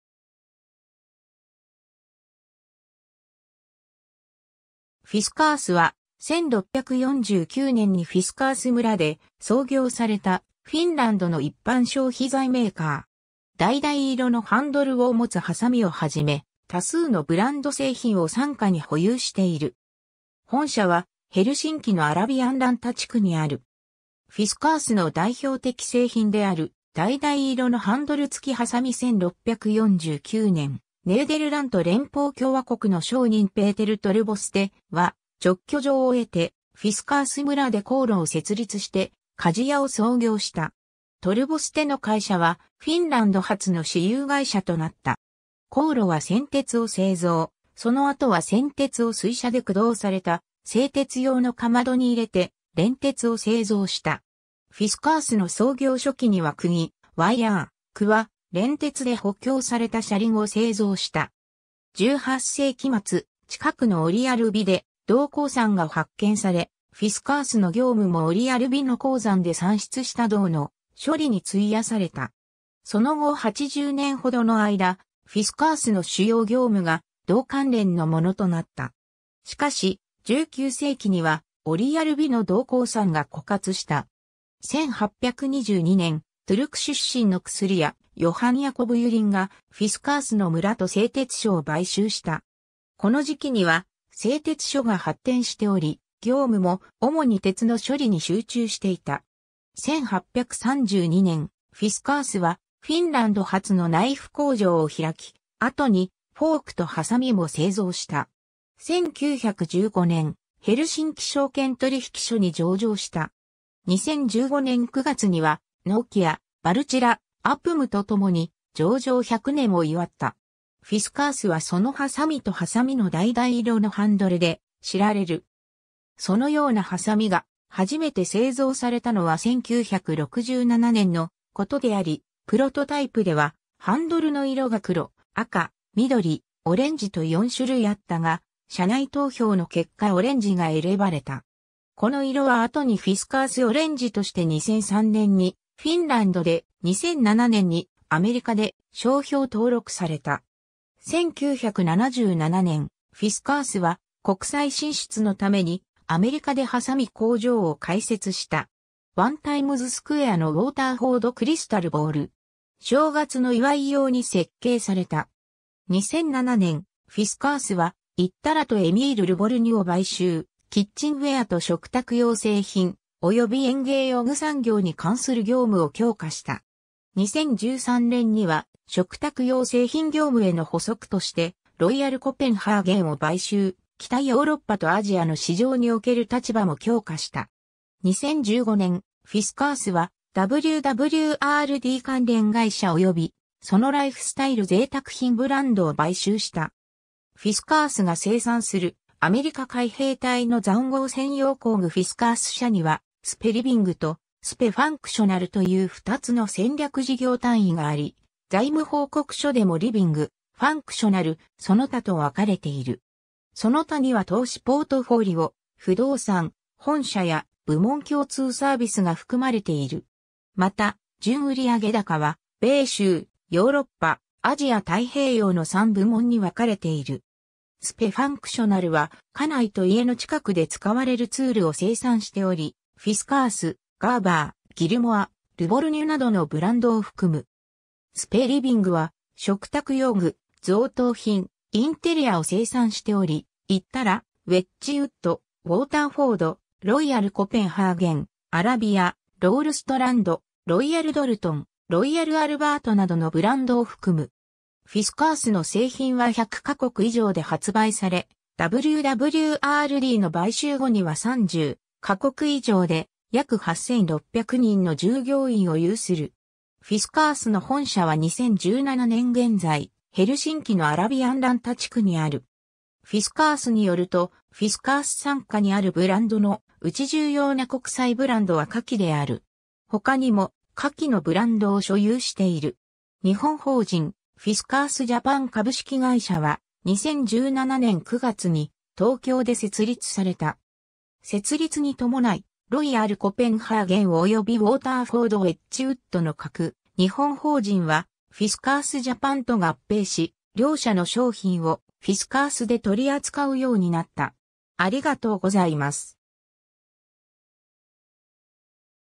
フィスカース
フィスカース（よりフィンランド語に近い表記ではフィルカルス、フィンランド語: Fiskars Oyj Abp）は、1649年にフィスカース村（現フィンランド領ラーセポリの一部、ヘルシンキの西約100km）で創業された、フィンランドの一般消費財メーカー。
橙色のハンドルを持つはさみをはじめ、多数のブランド製品を傘下に保有している。
本社はヘルシンキのアラビアンランタ地区にある。
 フィスカースの代表的製品である、橙色のハンドル付きはさみ 1649年、ネーデルラント連邦共和国の商人ペーテル・トルヴォステ（Peter Thorwöste）は勅許状を得て、フィスカース村で高炉を設立して鍛冶屋を創業した。
トルヴォステの会社はフィンランド初の私有会社となった。
高炉は銑鉄を製造、その後は銑鉄を水車で駆動された製鉄用のかまどに入れて錬鉄を製造した。
フィスカースの創業初期には釘、ワイヤー、鍬、錬鉄で補強された車輪を製造した。
 18世紀末、近くのオリヤルヴィ（Orijärvi）で銅鉱山が発見され、フィスカースの業務もオリヤルヴィの鉱山で産出した銅の処理に費やされた。
その後80年ほどの間、フィスカースの主要業務が銅関連のものとなった。
しかし、19世紀にはオリヤルヴィの銅鉱山が枯渇した。
 1822年、トゥルク出身の薬屋ヨハン・ヤコブ・ユリン（Johan Jacob Julin）がフィスカースの村と製鉄所を買収した。
この時期には製鉄所が発展しており、業務も主に鉄の処理に集中していた。
1832年、フィスカースはフィンランド初のナイフ工場を開き、後にフォークとはさみも製造した。
 1915年、ヘルシンキ証券取引所（現：ナスダック・ヘルシンキ）に上場した。
2015年9月にはノキア、バルチラ、UPMとともに上場100年を祝った。
 フィスカースはそのはさみとはさみの橙色のハンドルで知られる。
そのようなはさみがはじめて製造されたのは1967年のことであり、プロトタイプではハンドルの色が黒、赤、緑、オレンジと4種類あったが、社内投票の結果オレンジが得ればれた。
この色は後にフィスカース・オレンジとして2003年にフィンランドで、2007年にアメリカで商標登録された。
1977年、フィスカースは国際進出のためにアメリカではさみ工場を開設した。
 ワン・タイムズスクエアのウォーターフォード・クリスタル・ボール。
正月の祝い用に設計された。
 2007年、フィスカースはイッタラとエミール・ルボルニュを買収、キッチンウェアと食卓用製品、および園芸用具産業に関する業務を強化した。
2013年には食卓用製品業務への補足としてロイヤルコペンハーゲンを買収、北ヨーロッパとアジアの市場における立場も強化した。
2015年、フィスカースはWWRD関連会社およびそのライフスタイル贅沢品ブランド（ウォーターフォード、ウェッジウッド、ロイヤルドルトン、ロイヤルアルバート、ロガシュカなど）を買収した。
 フィスカースが生産する、アメリカ海兵隊の塹壕戦用工具 フィスカース社にはSBUリビングとSBUファンクショナルという2つの戦略事業単位（SBU）があり、財務報告書でもリビング、ファンクショナル、その他と分かれている。
「その他」には投資ポートフォリオ、不動産、本社や部門共通サービスが含まれている。
また、純売上高は米州、ヨーロッパ、アジア太平洋の3部門に分かれている。
 SBUファンクショナルは家内と家の近くで使われるツールを生産しており、フィスカース、ガーバー、ギルモア、ルボルニュなどのブランドを含む。
SBUリビングは食卓用具、贈答品、インテリアを生産しており、イッタラ、ウェッジウッド、ウォーターフォード、ロイヤルコペンハーゲン、アラビア、ロールストランド、ロイヤルドルトン、ロイヤル・アルバートなどのブランドを含む。
 フィスカースの製品は100か国以上で発売され、WWRDの買収後には30か国以上で約8,600人の従業員を有する。
 フィスカースの本社は2017年現在、ヘルシンキのアラビアンランタ地区にある。
 フィスカースによると、フィスカース参加にあるブランドのうち重要な国際ブランドは下記である。
 他にも下記のブランドを所有している。
 日本法人「フィスカースジャパン株式会社」は、2017年9月に東京（千代田区）で設立された。
設立に伴い、ロイヤルコペンハ...